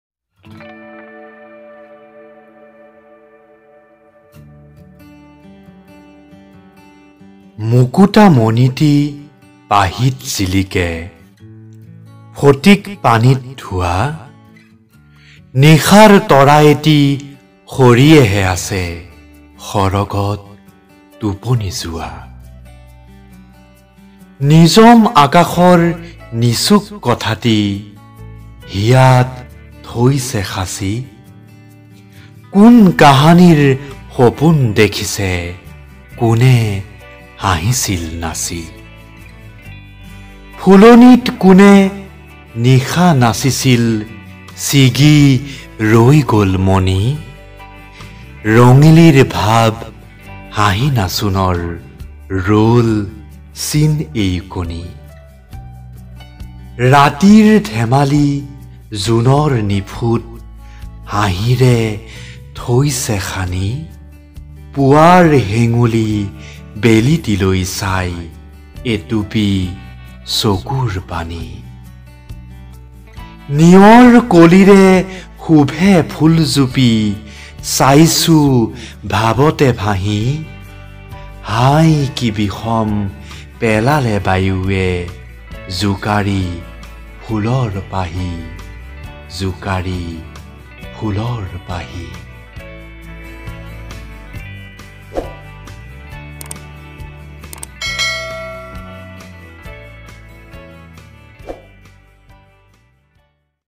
Niyor Chandrakumar Agarwala নিয়ৰ কবি চন্দ্ৰকুমাৰ আগৰৱালা Class Eight Standard ChildPoem অসমীয়া কবিতা
কবিতা- নিয়ৰ
কবি- চন্দ্ৰকুমাৰ আগৰৱালা
পাঠ- প্ৰৱাল গোস্বামী

Poem- Niyar
Poet- Chandra Kumar Agarwala
Recited By- Prabal Goswami

মুকুতা মণিটি পাহিত জিলিকে
ফটিক পানীত ধোৱা
নিশাৰ তৰা এটি সৰিয়েহে আছে
সৰগত টোপনি যোৱা

নিজম আকাশৰ নিচুক কথাটি
হিয়াত থৈছে সাঁচি
কোন কাহানিৰ সপোন দেখিছে
কোনে হাঁহিছিল নাচি
Mukuta Moniti Pahit Jilike
Fatik Panit Dhua
Nishar Tora Eti Sariyehe Ase
Saragat Tuponi Jua

Nijom Akashor Nisuk Kothati
Hiyat Thoise Sasi
Kon Kahanir Sapun Dekhise
Kone Hahisil Nasi.
(সম্পূৰ্ণ কবিতাটো পঢ়িবলৈ তলৰ লিংকটোত ক্লিক কৰক)

©অসমীয়া কবিতা আবৃত্তি©
Assamese Poem Recitation
Prabal Goswami Poem Recitation

আমাৰ চেনেলৰ কবিতাৰ Playlists__

আমাৰ ভ্ৰমণ আৰু অনুষ্ঠানৰ কিছু ভিডিঅ’ৰ লিংক….